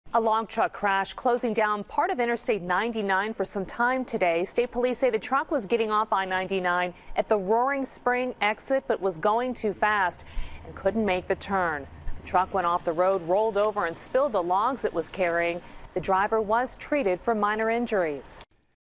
Log truck rolls over while exiting Blair Co. highway
FREEDOM TOWNSHIP, Pa. -- A log truck overturned in Blair County, spilling its timber and forcing the closure of a highway offramp.State police at Hollidaysburg said Marlin Kauffman, 42, was traveling too fast while exiting Interstate 99 south at the Roaring Spring interchange (Exit 23) at about 7:15 a.m. Monday, causing him to lose control of his truck and roll it onto its side.Police said Kauffman suffered minor injuries in the crash and that he was wearing his seat belt.Another truck was brought in to transfer the logs, officials said. No other vehicles were involved in the crash.